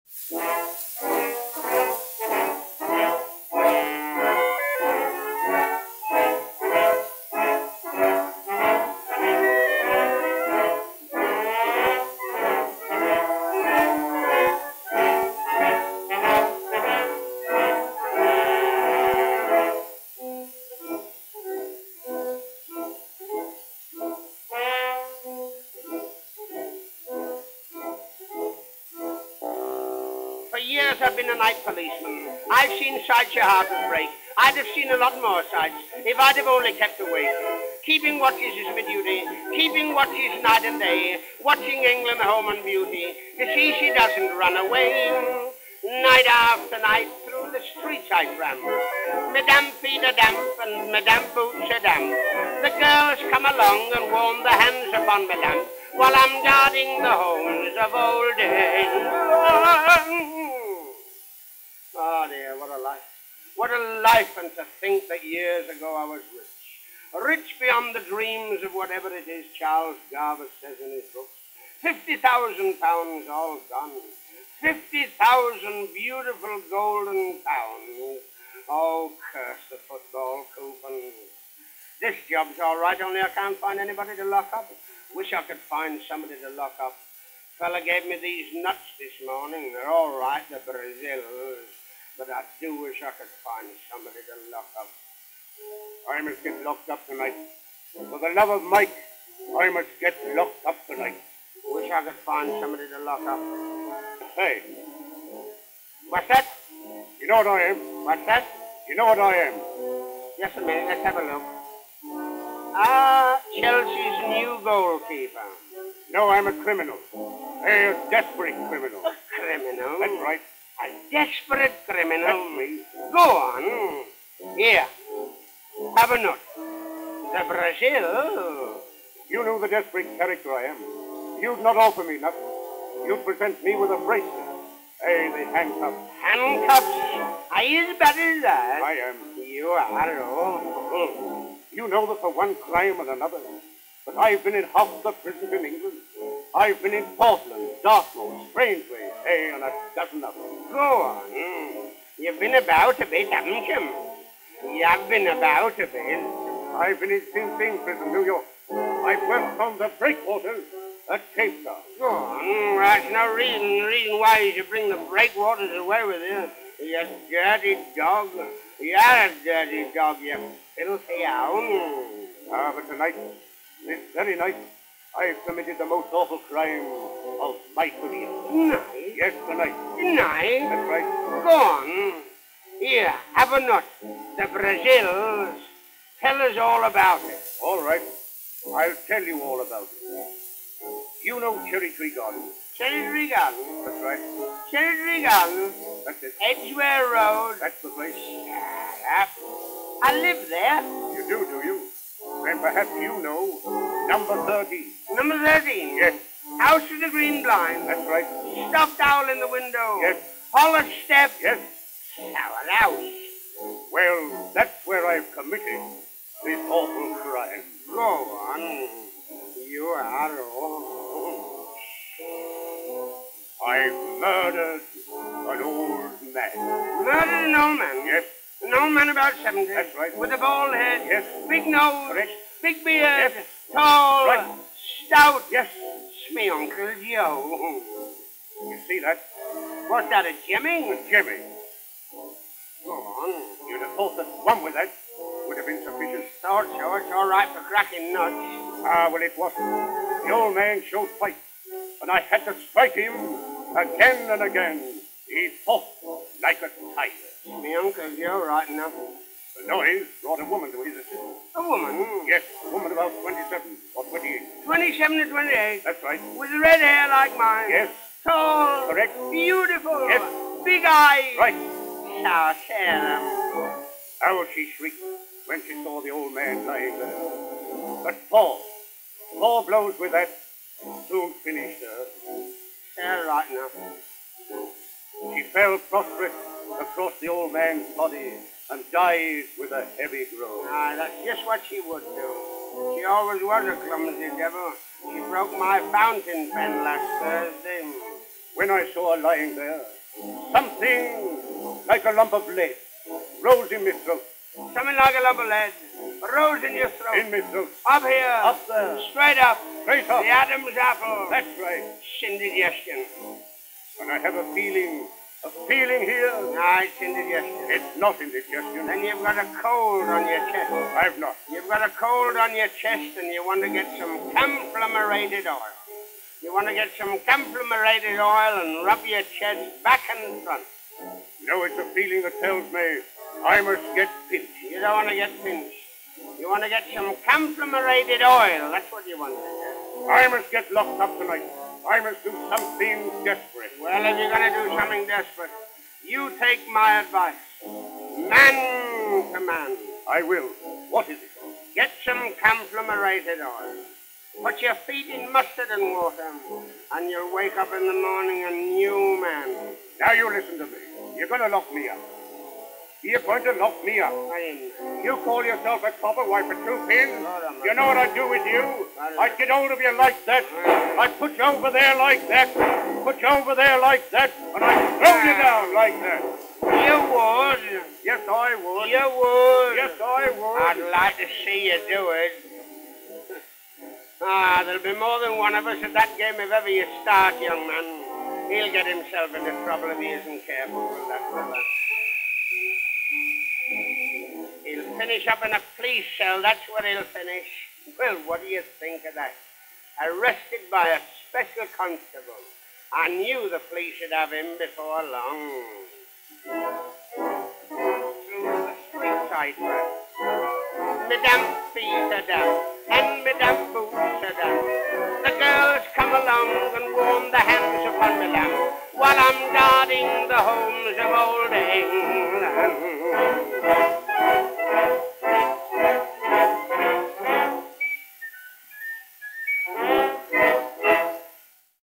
Harry Weldon: 'The Policeman' (1920)
Don't expect sophisticated comedy. Harry Weldon began his career in Fred Karno's knockabout comic music-hall team, alongside Stan Laurel and Charlie Chaplin. He later broke out on his own with routines such as 'The Policeman', in a style which later inspired Sandy Powell to develop his own act. Recorded 8 July 1920.